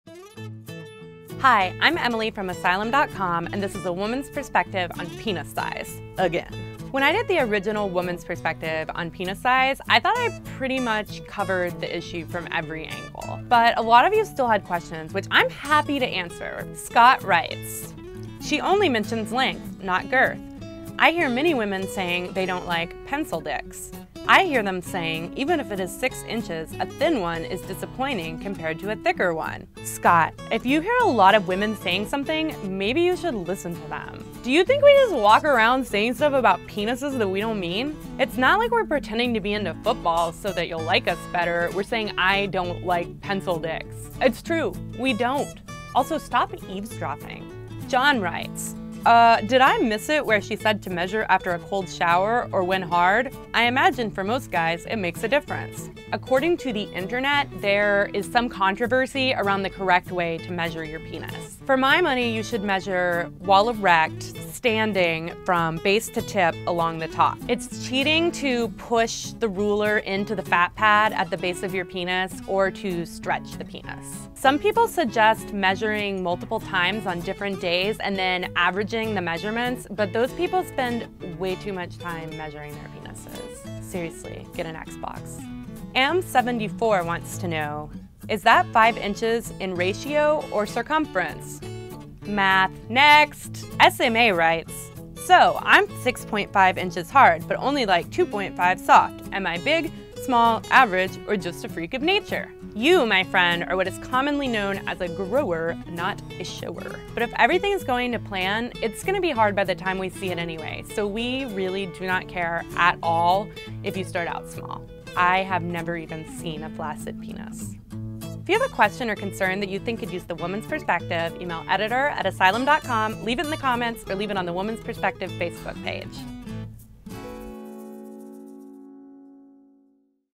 A Woman's Perspective on Penis Size, Part 2
In this hard-hitting follow-up to the first Woman's Perspective on penis size, Asylum's token girl, Emily McCombs, answers additional questions on the all-important topic of penis size.